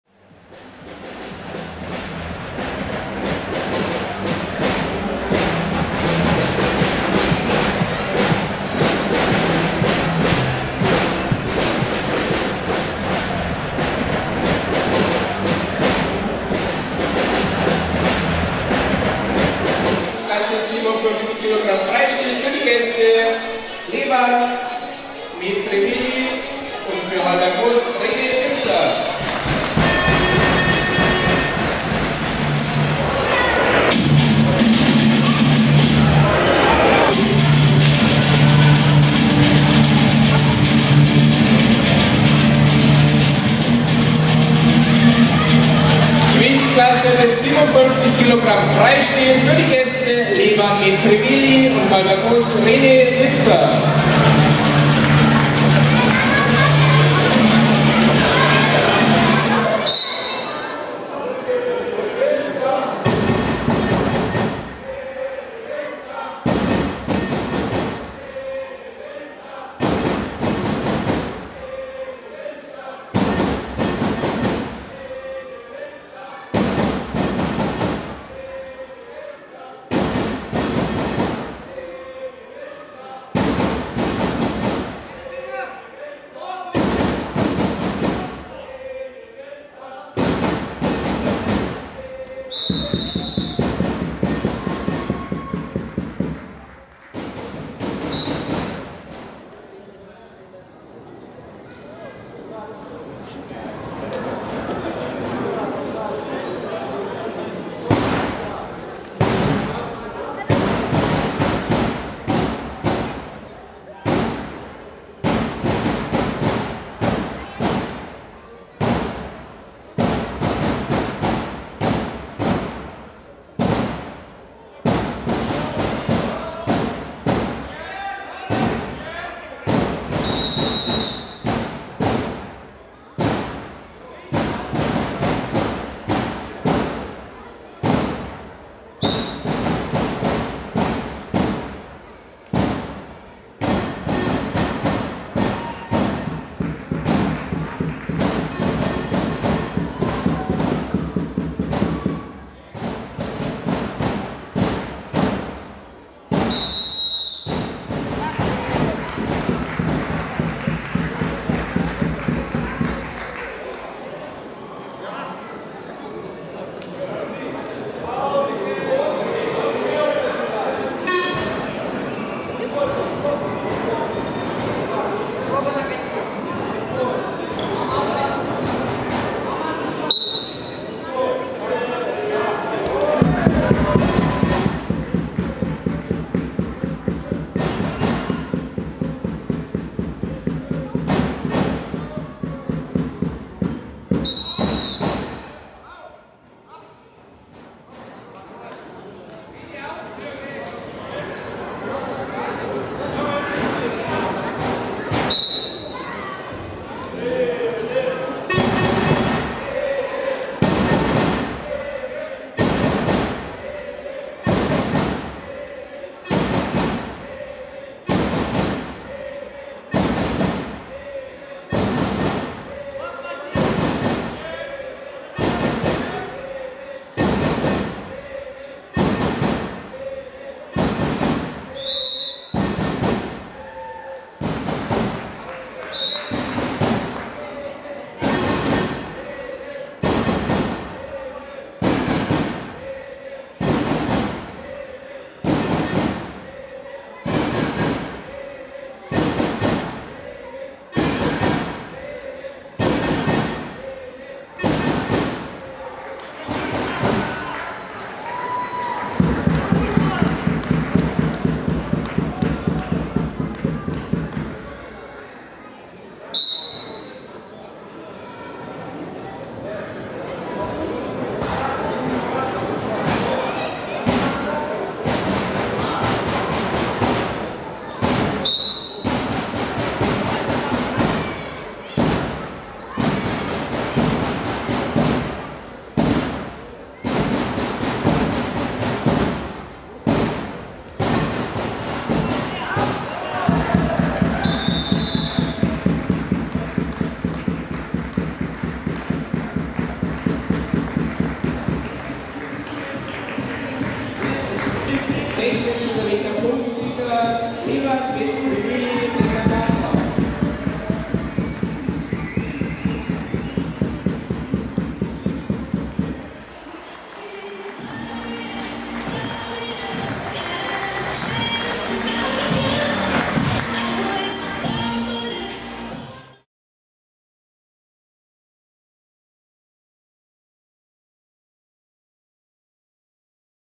RINGEN | Hallbergmoos (Winter, R.) - Neckargartach (Vartanov, L. M.) 57kg Freistil
DRB Bundesliga PlayOffs 1/8 Finale Vorkampf
SV Siegfried Hallbergmoos - VfL Neckargartach
WINTER, R. vs. VARTANOV, L. M.
0:4 (TÜ) / TP 0:15 / [time 02:50]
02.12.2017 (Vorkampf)